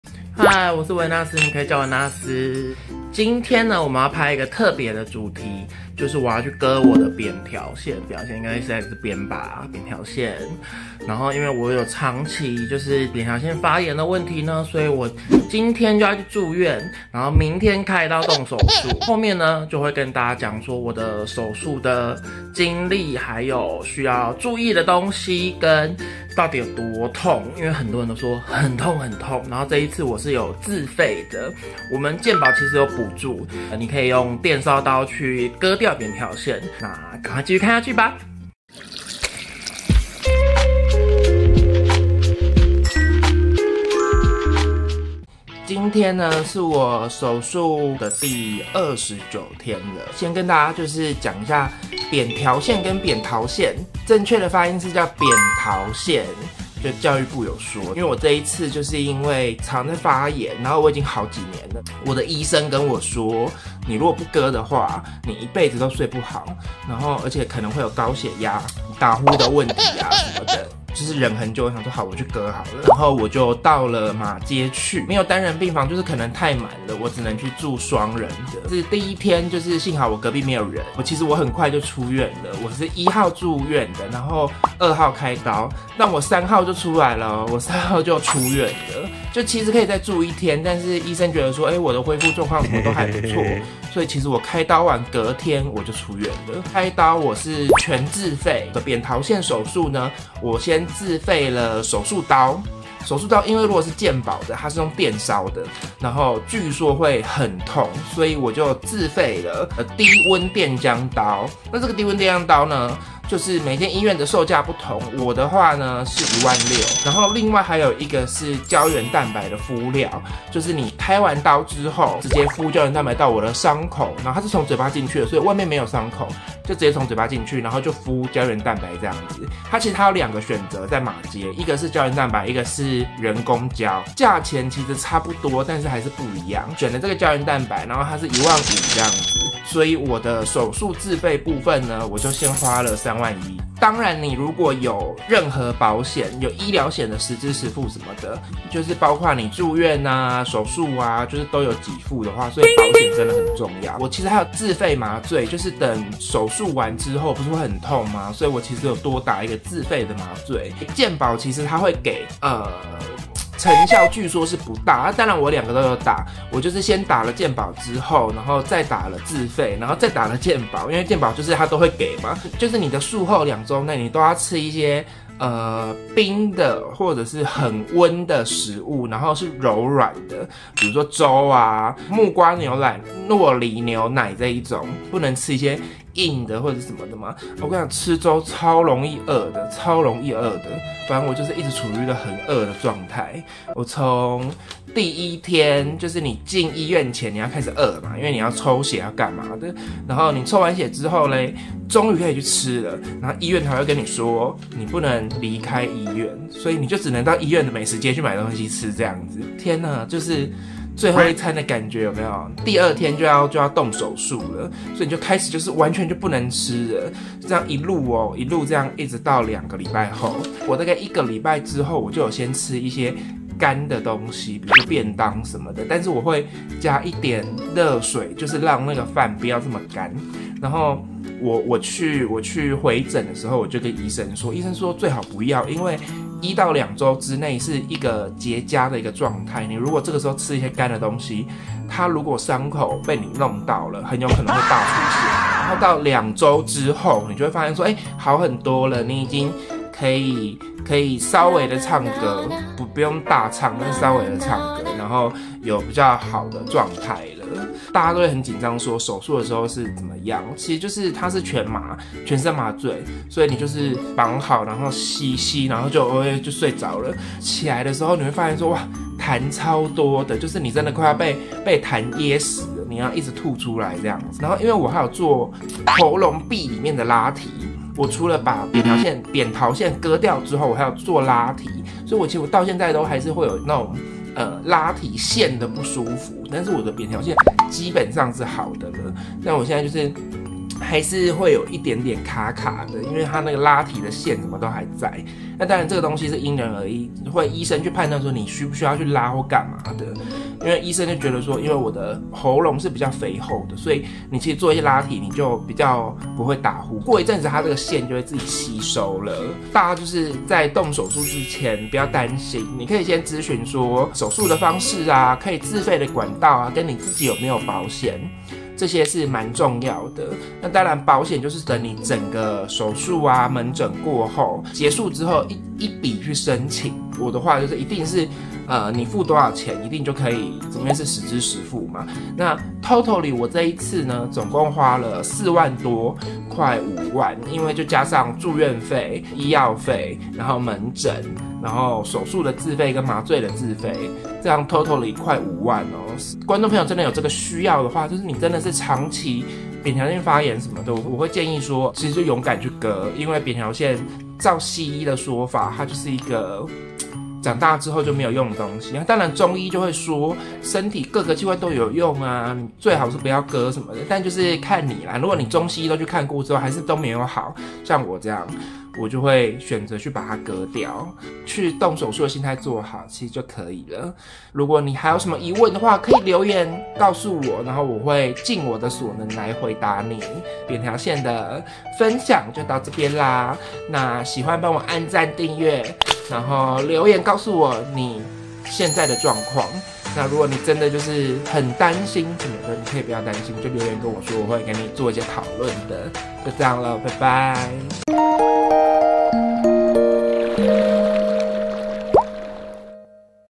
我去割了扁桃腺！！！ 全身麻醉還要住院！｜Tonsillitis手術經驗分享｜納斯想怎樣
我去割了
全自費但沒有打嗎啡，我用的是一般的自費止痛，加上健保的消炎止痛點滴。
如果你也在煩腦要不要去割扁桃腺，趕快留言跟我分享吧～